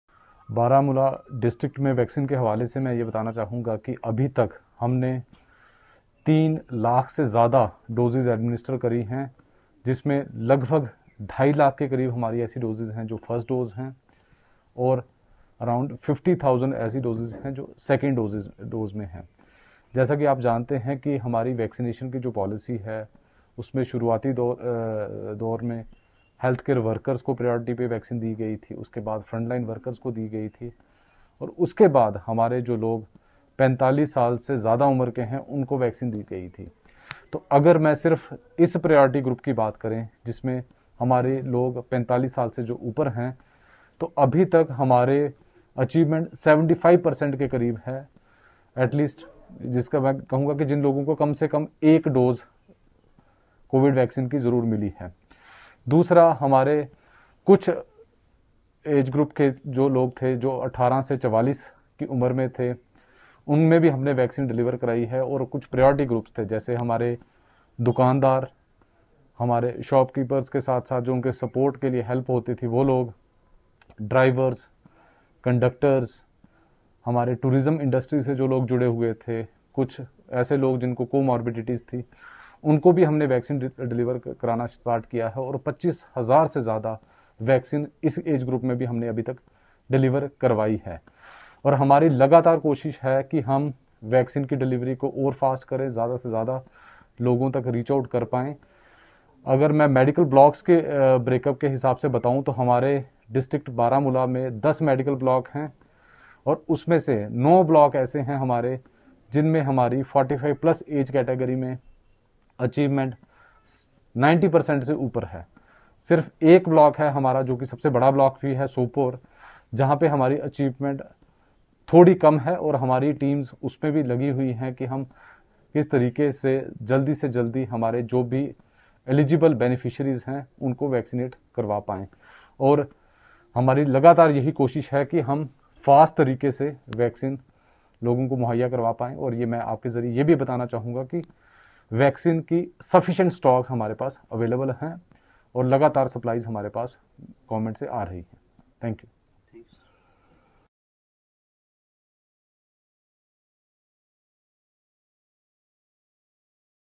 90% Above 45-yr In 9 Blocks Vaccinated: DC Baramulla
| Excelsior News |

90% Above 45-yr In 9 Blocks Vaccinated: DC Baramulla